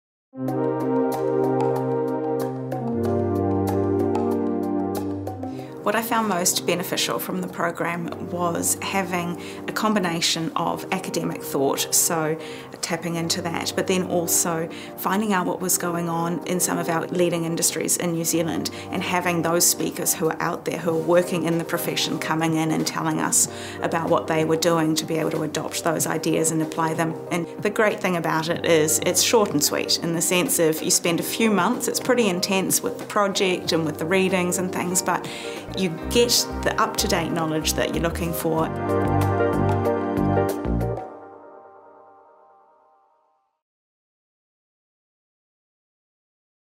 Reboot your people strategy for a changing world
Executive and Professional Development’s Transitioning to C-Suite Programme – Strategic GM and People specialisation prepares mid-career professionals to step up into a leadership role. For Frankie Ifrah, National People & Performance Manager for Brian Perry Civil at Fletcher Building, the combination of academic thought, real-world application of concepts, and industry networking offered by the programme has been a value-add to her life professionally and personally. “The programme is short and sweet - and intense in terms of projects and readings - but you get the up-to-date knowledge you’re looking for.”